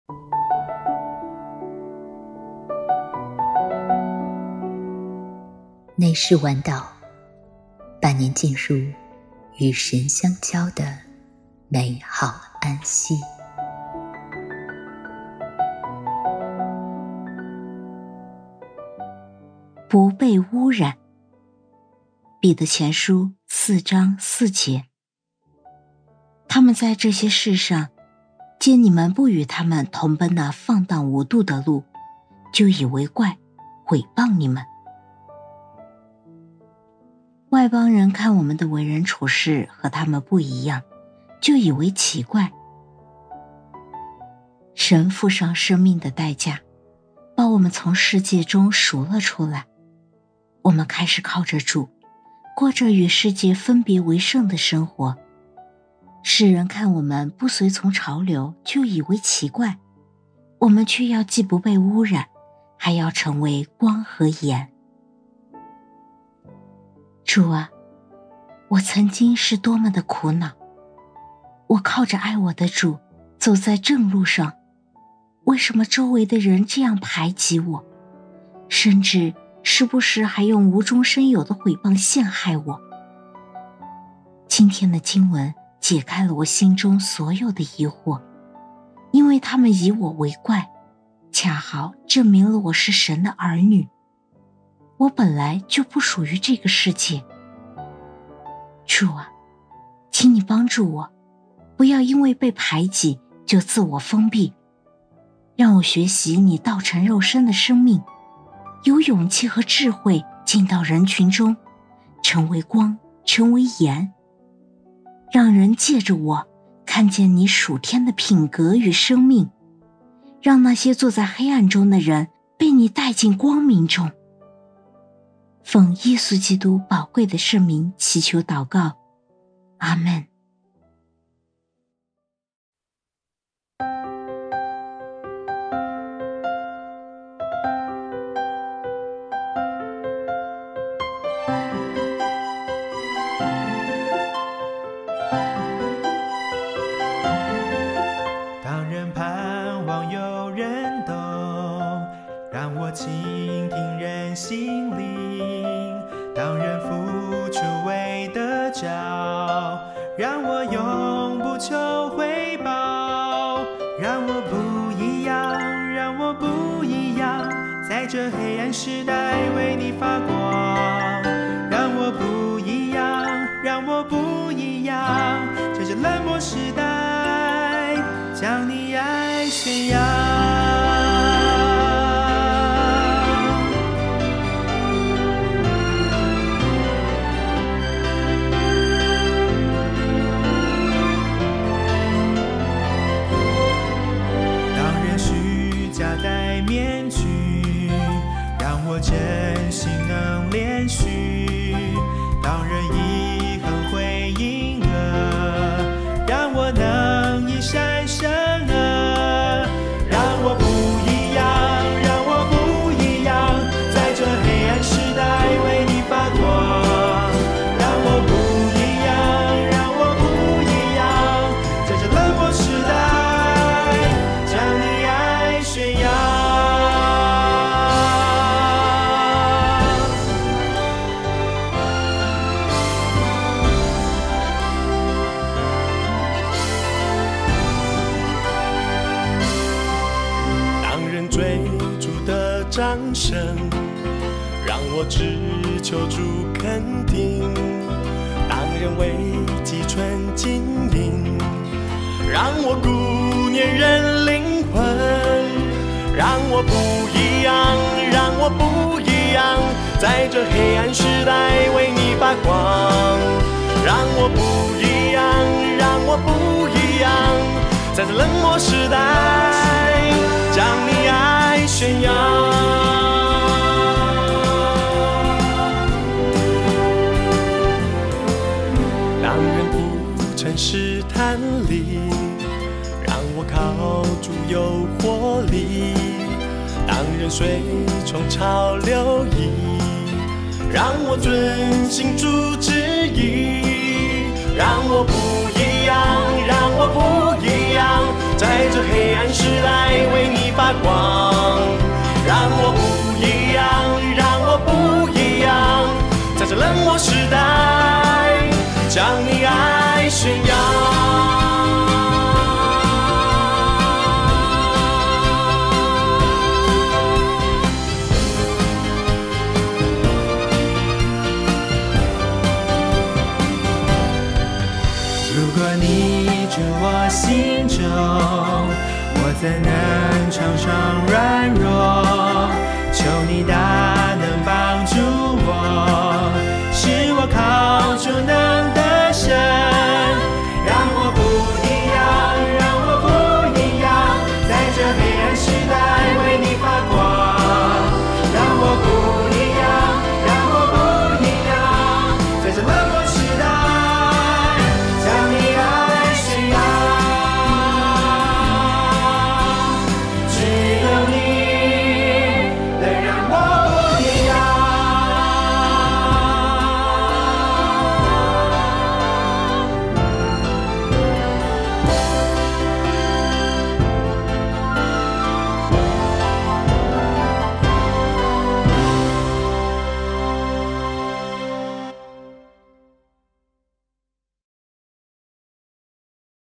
内室晚祷#396｜不被污染｜不随从世界的潮流|成为光和盐|祷告求神让我学习道成肉身的生命
经文
【彼前4:4】他们在这些事上，见你们不与他们同奔那放荡无度的路，就以为怪，毁谤你们。

观察
外邦人看我们的为人处世，和他们不一样，就以为奇怪。

应用
神付上生命代价，把我们从世界中赎了出来，我们开始靠着主，过着与世界分别为圣的生活。世人看我们不随从潮流就以为奇怪，我们却要既不被污染，还要成为光和盐。

祷告
主啊，我曾经是多么的苦恼，我是靠着爱我的主，走在正路上，为什么周围的人，这样排挤我，甚至时不时还用无中生有的毁谤陷害我。今天的经文，解开了我心里所有的疑惑，因为他们以我为怪，恰好证明了我是神的儿女，我本来就不属于这个世界！主啊，也请你帮助我不要因为被排挤就自我封闭，让我学习你道成肉身的生命，有勇气和智慧进到人群中，成为光，成为盐，让人籍着我看见你属天的品格与生命，让那些坐在黑暗中的人，被你带进光明中！奉耶稣基督宝贵的圣名祈求祷告！阿们！